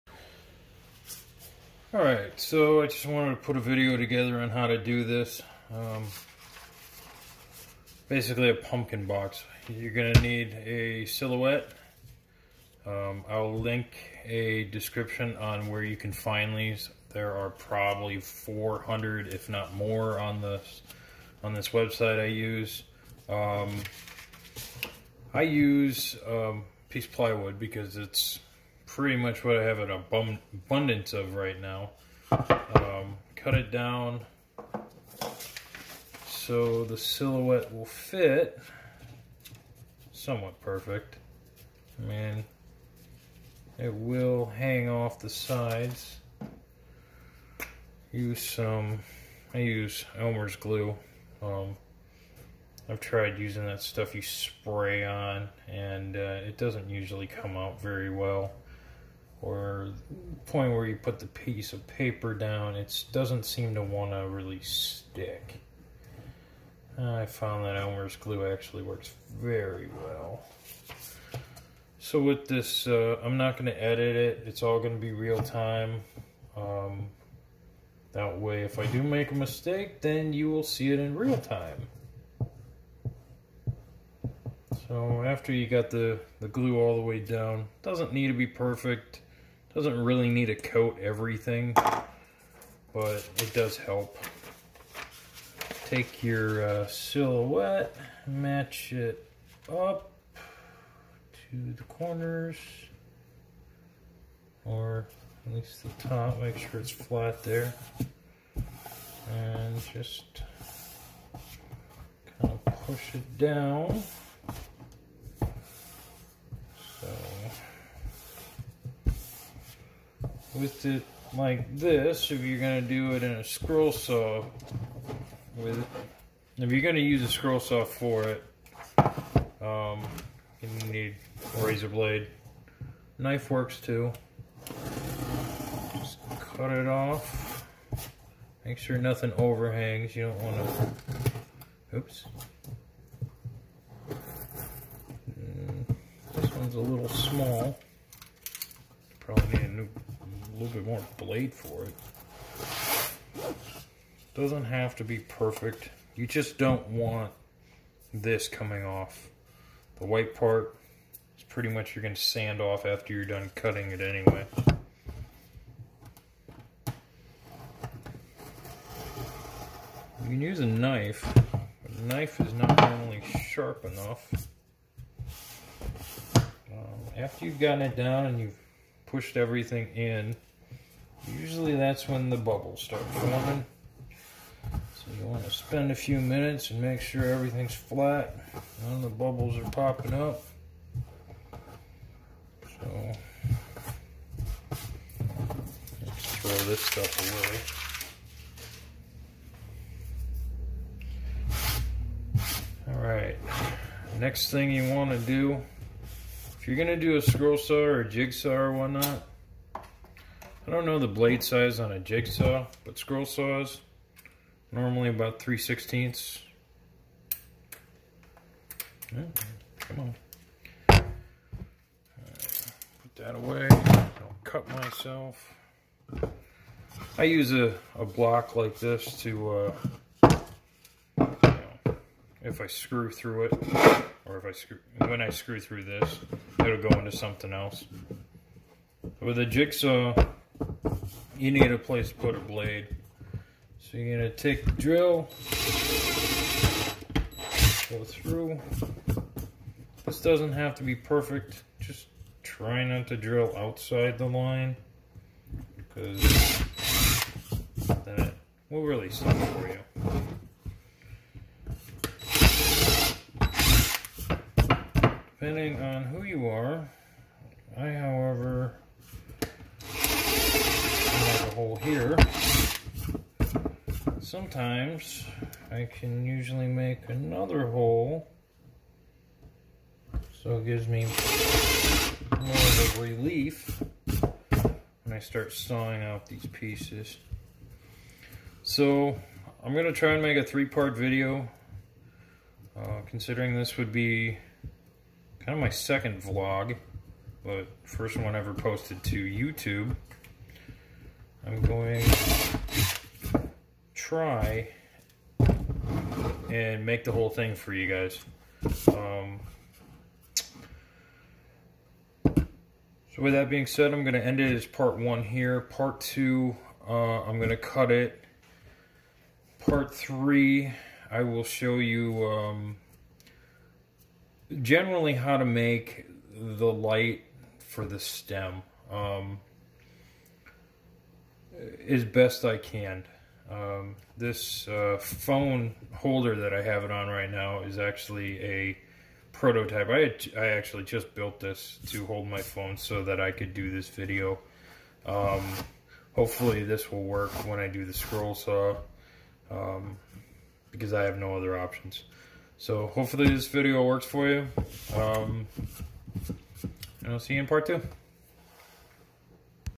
This Is How to make a Pumpkin Box Part 1
This is Part 1 of a mini series of how to make a Pumpkin Box to decorate your porch for Halloween.

This is where I found the stencils to make the boxes